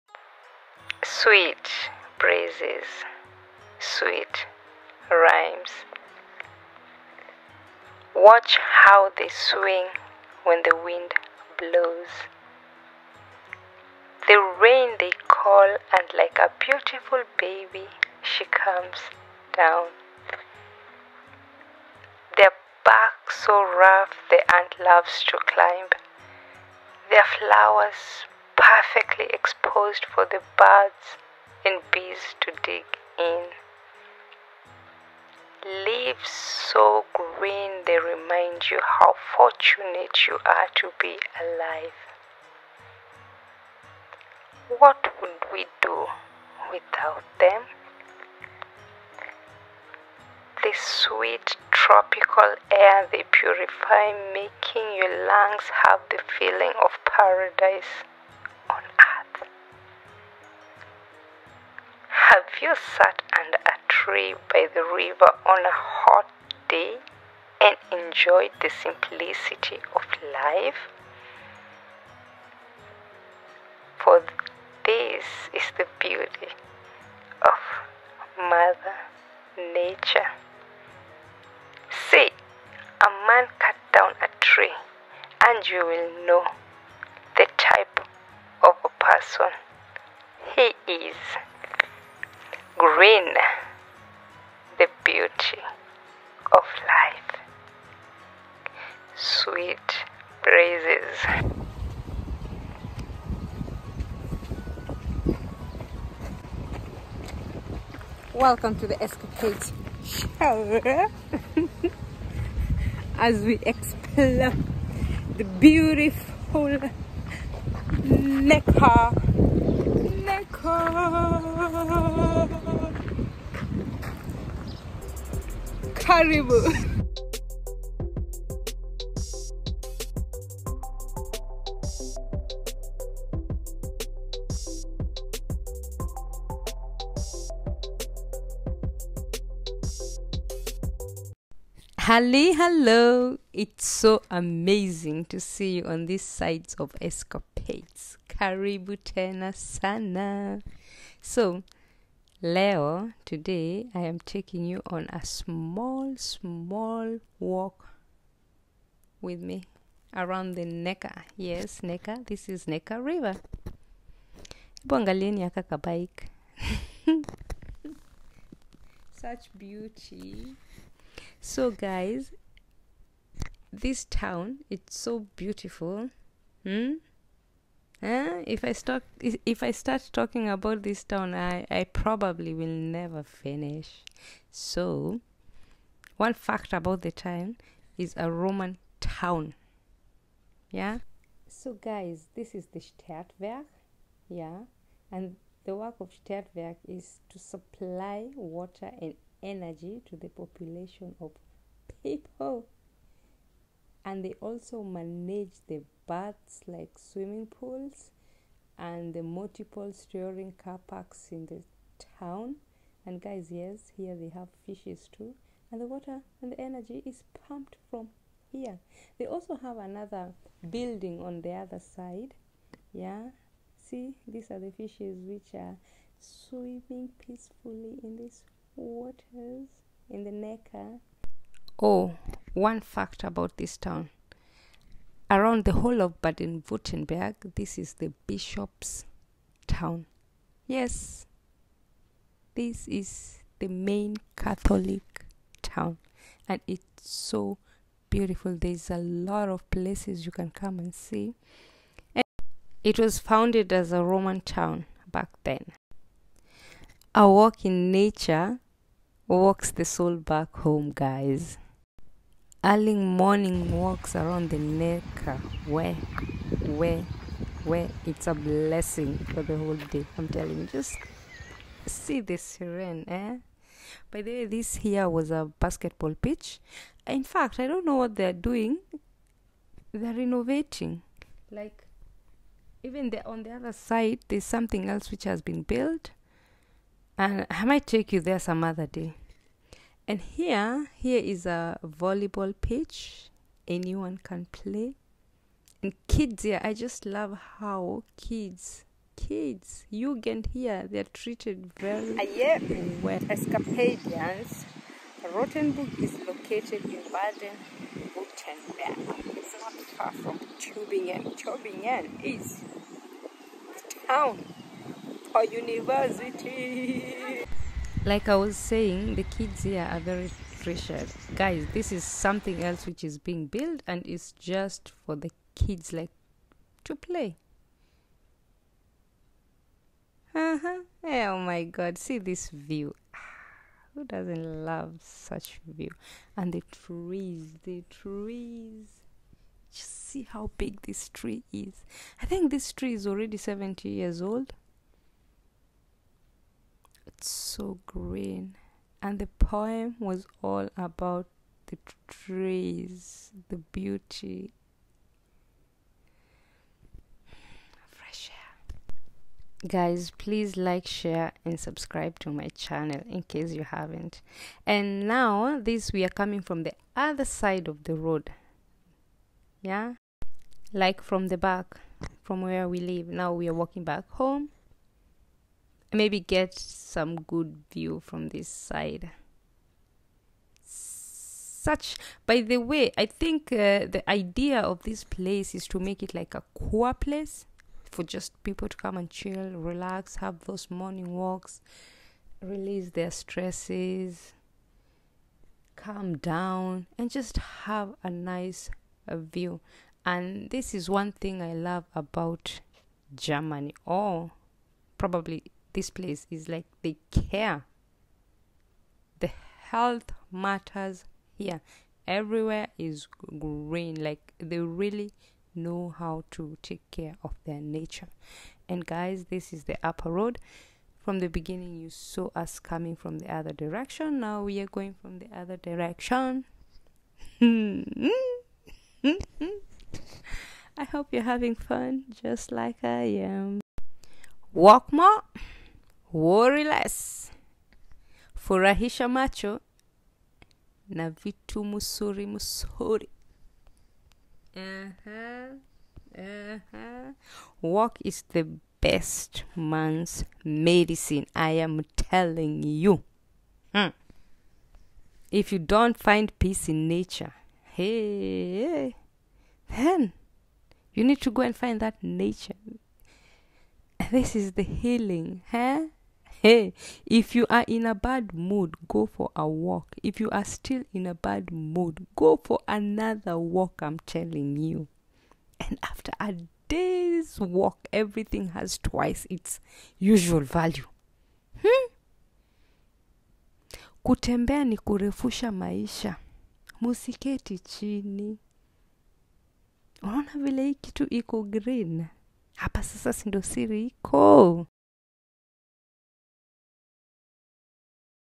Poetry and nature a man’s Best Friend
Jambo!

Welcome to this new good vibes segment of Escapades, this time  we are embarking on
 amazing adventure , here I walk with you around the Neckar river.
Watch the beautiful serenity as we find peace and calmness where we are.